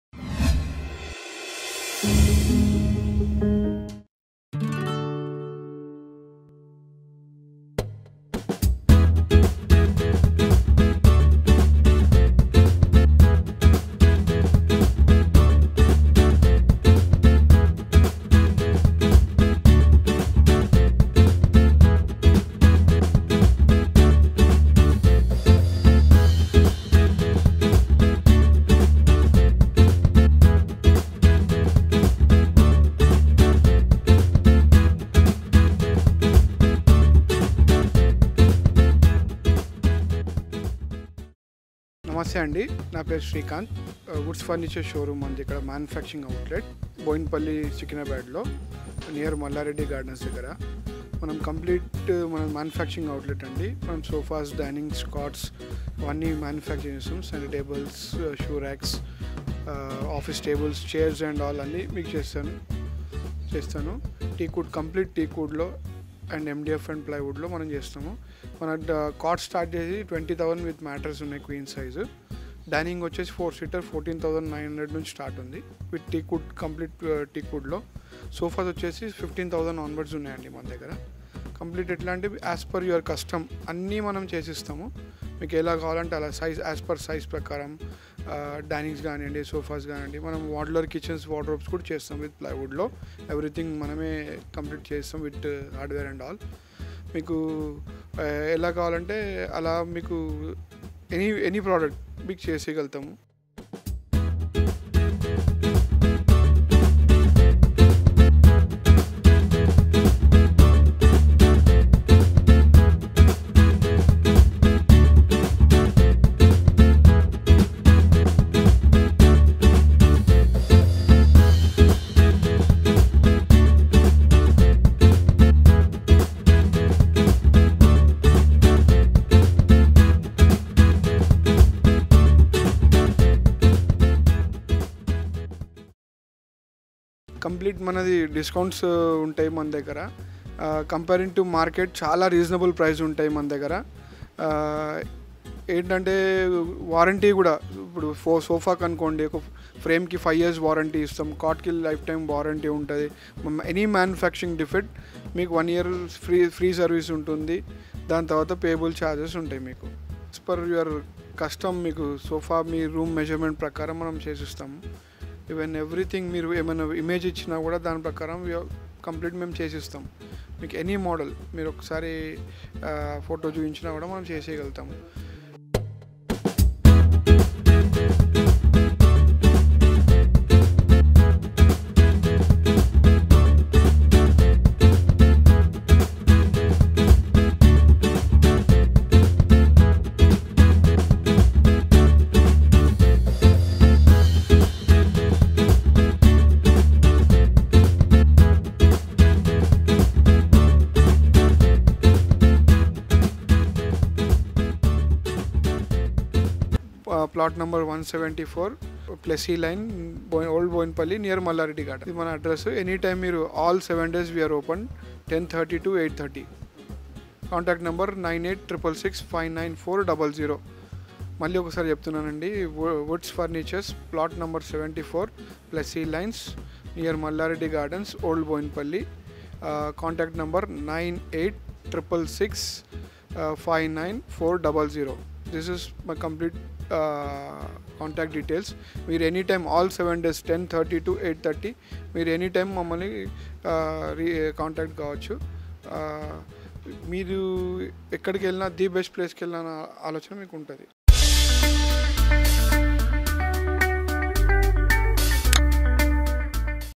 "At Woods Furniture, we turn your house into a home with our quality designs, at Old Bowenpally."
Find top-quality furniture at Woods Furniture, Old Bowenpally. Have a look at this video to see the range of tables, beds, sofas, and other products being sold at their showroom. Listen to the owner give you details about the items available and their pricing, and head over to their store to go furniture shopping!